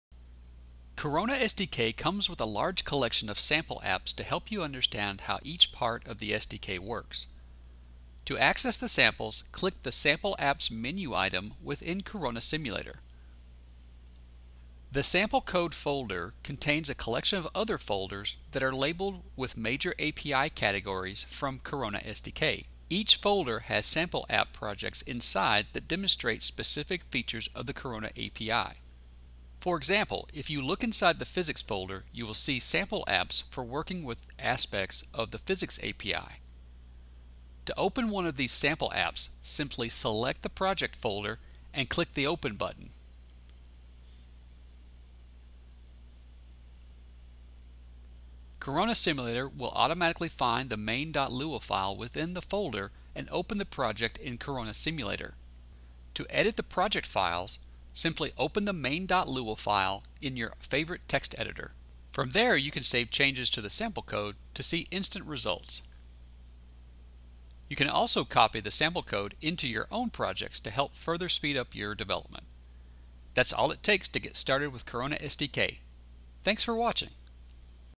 Start Coding with Corona SDK
Corona SDK comes with a large collection of Sample Apps to help you understand how each part of the SDK works.